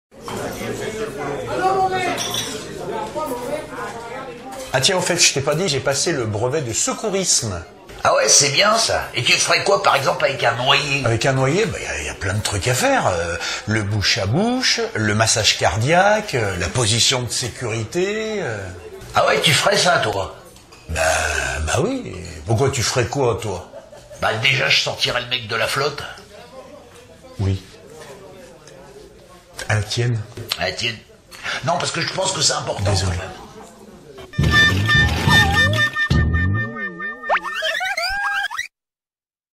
Brèves de comptoir - Le Brevet de Secourisme
Dans notre bistro lyonnais, on parle, aujourd'hui, du Brevet de Secourisme.

Merci d'avance  ;-)